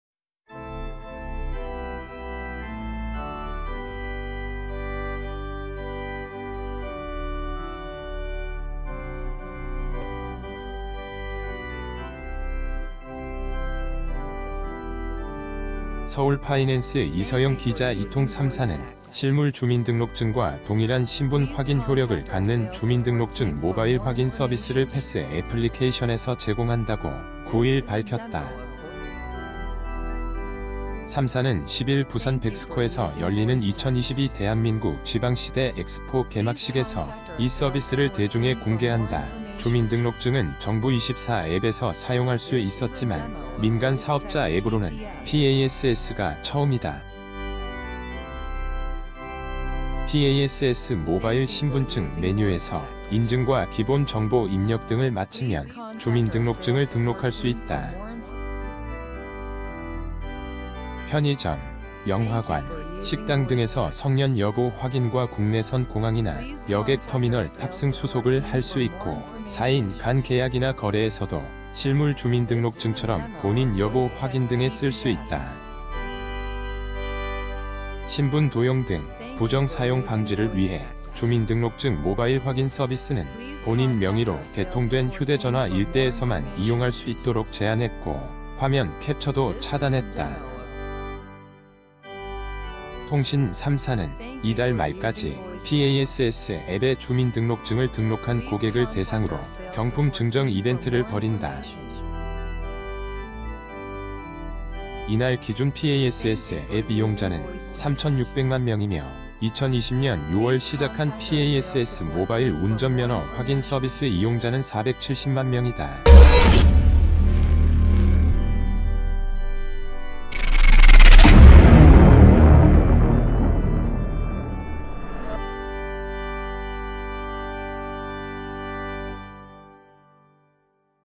"주민증 들고다닐 필요없다" 통신3사 PASS앱에서 확인 서비스
"주민증 들고다닐 필요없다" 통신3사 PASS앱에서 확인 서비스 |Invicta Men's Pro Diver Stainless Steel Quartz Watch . Please help me with an order to maintain the system, thanks a lot.| [서울파이낸스 이서영 기자] 이통 3사(SK텔레콤·KT·LG유플러스)는 실물 주민등록증과 동일한 신분 확인 효력을 갖는 '주민등록증 모바일 확인 서비스'를 패쓰(PASS) 애플리케이션에서 제공한다고 9일 밝혔다. 3사는 10일 부산 벡스코에서 열리는 2022 대한민국 지방시대 엑스포 개막식에서 이 서비스를 대중에 공개한다 주민등록증은 정부24 앱에서 사용할 수 있었지만, 민간 사업자 앱으로는 PASS가 처음이다. PASS '모바일 신분증' 메뉴에서 인증과 기본 정보 입력 등을 마치면 주민등록증을 등록할 수 있다. 편의점, 영화관, 식당 등에서 성년 여부 확인과 국내선 공항이나 여객 터미널 탑승 수속을 할 수 있고, 사인 간 계약이나 거래에서도 실물 주민등록증처럼 본인 여부 확인 등에 쓸 수 있다. 신분 도용 등 부정 사용 방지를 위해 주민등록증 모바일 확인 서비스는 본인 명의로 개통된 휴대전화 1대에서만 이용할 수 있도록 제한했고 화면 캡처도 차단했다. 통신 3사는 이달 말까지 PASS 앱에 주민등록증을 등록한 고객을 대상으로 경품 증정 이벤트를 벌인다. 이날 기준 PASS 앱 이용자는 3600만명이며 2020년 6월 시작한 PASS '모바일 운전면허 확인 서비스' 이용자는 470만명이다.